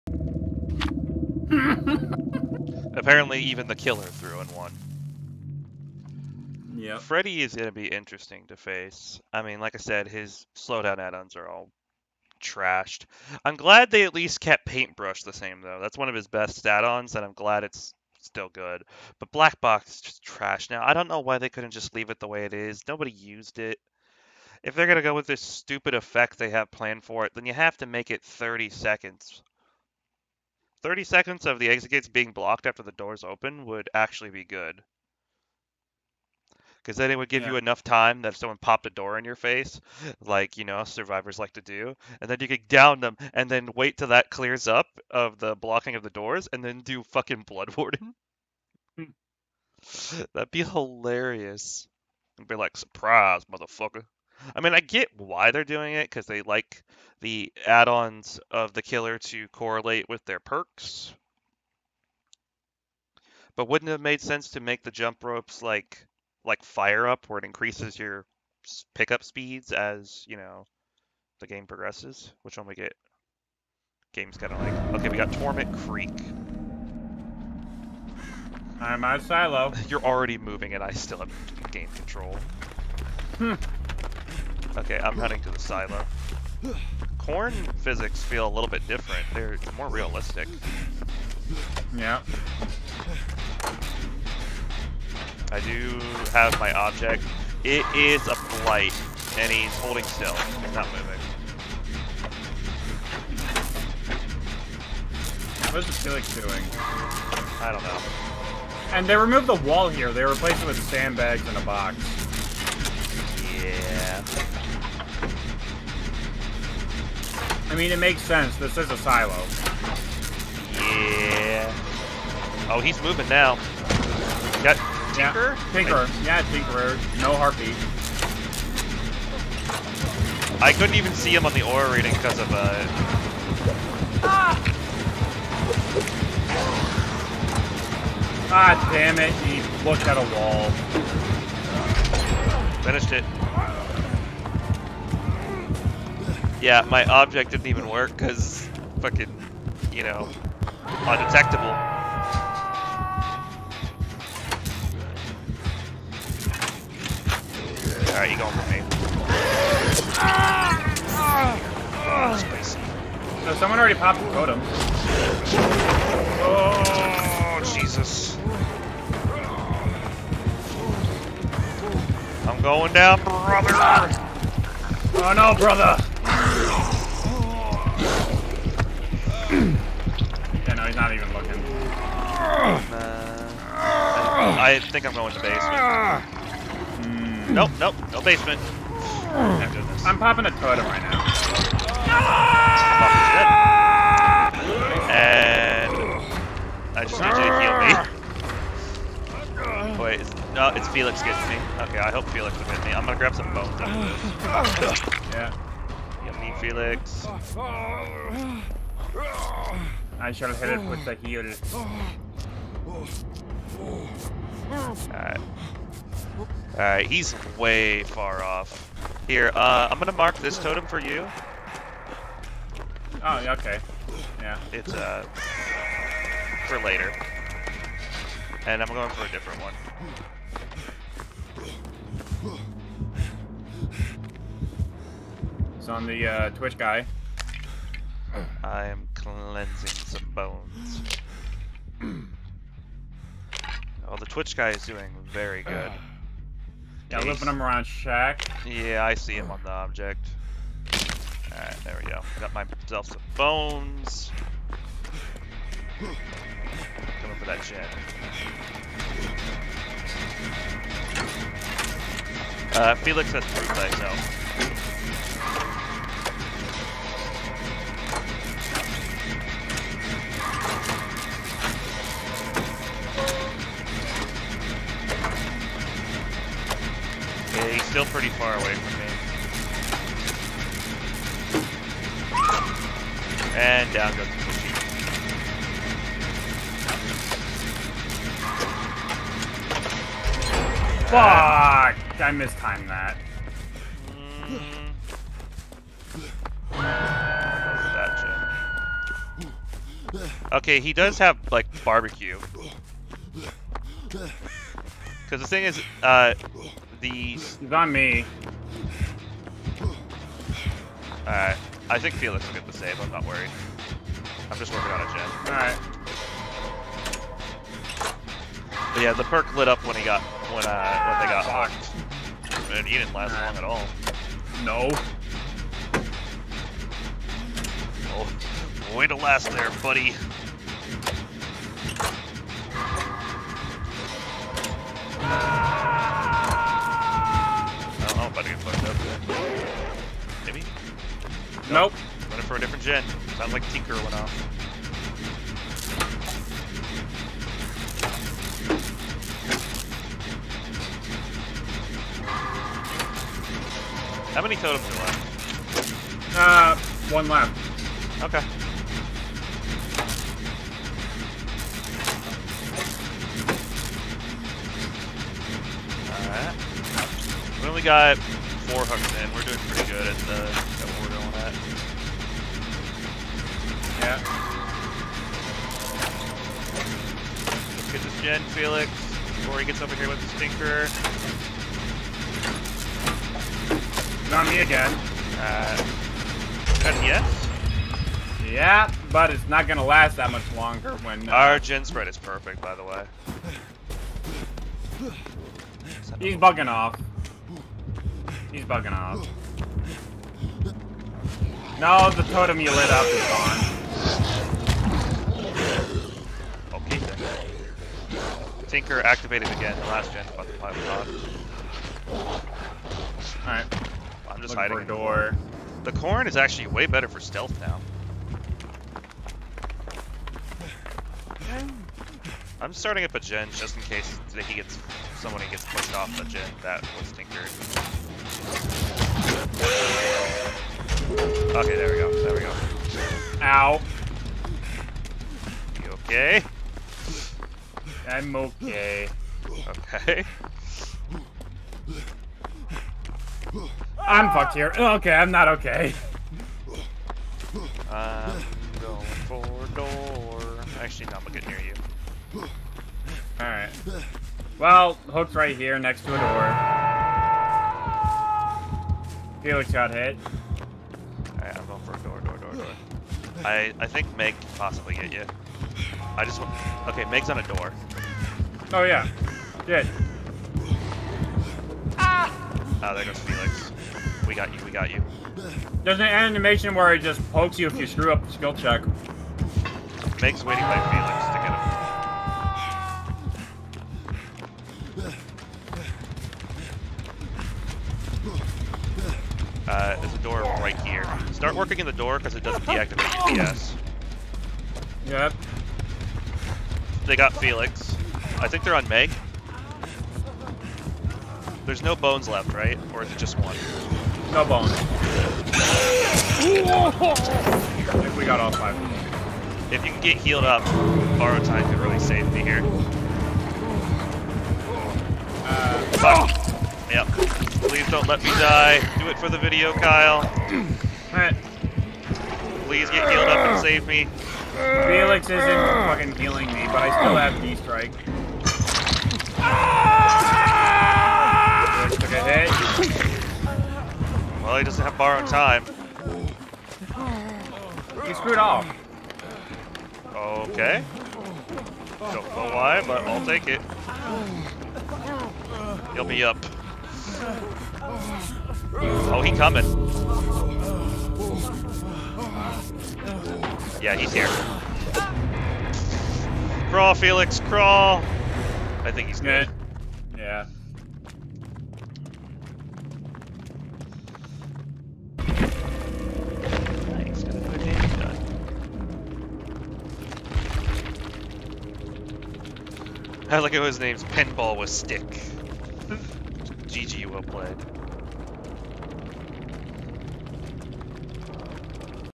First Match on Updated Torment Creek
I really like the changes to this map. The corn is so much better for stealth gameplay now. I hope you enjoy the video.